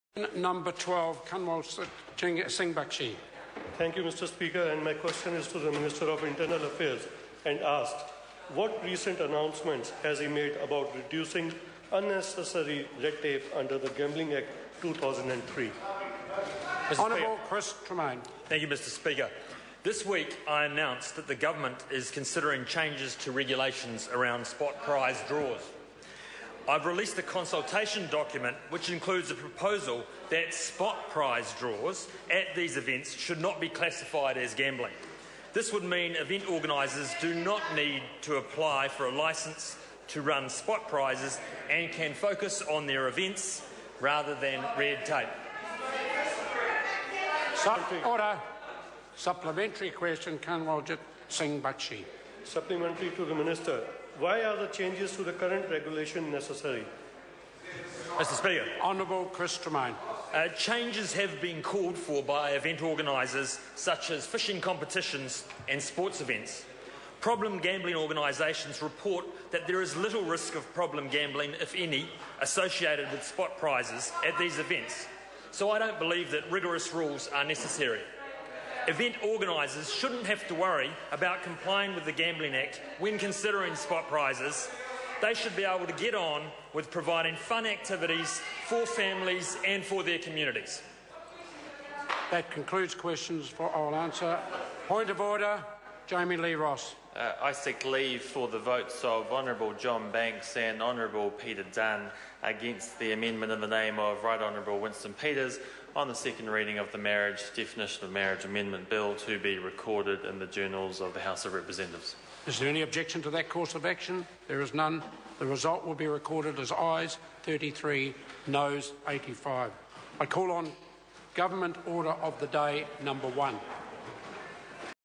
14.3.13 - Question 12: Kanwaljit Singh Bakshi to the Minister of Internal Affairs
What recent announcements has he made about reducing unnecessary red tape under the gambling act 2003?